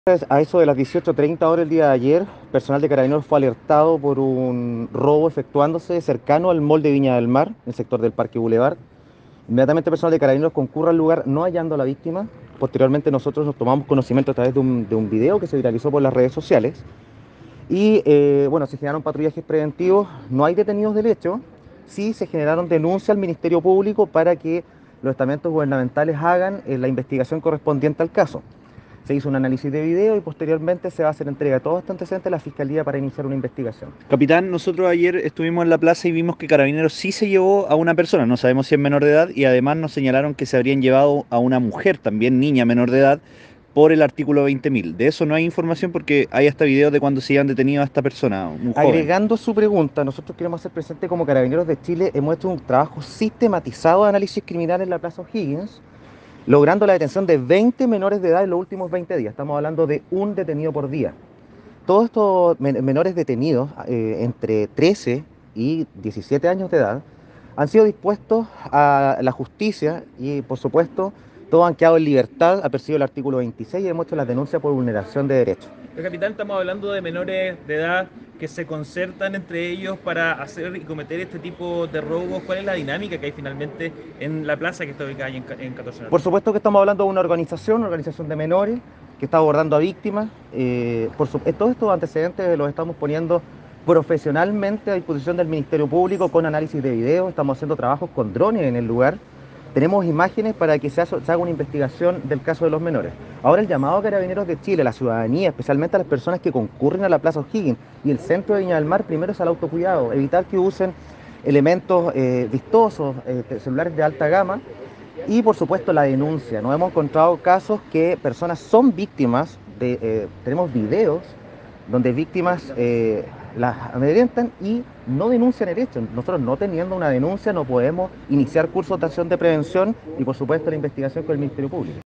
Asalto en boulevard viñamarino: aún sin detenidos, pero 20 menores han sido capturados en el sector
Sin detenidos se mantienen las diligencias derivadas del violento asalto que se registró ayer por la tarde en el frontis del Boulevar Marina, y que afectó a una pareja de turistas argentinos, los que fueron atacados por seis jóvenes delincuentes, presumiblemente, todos menores de edad.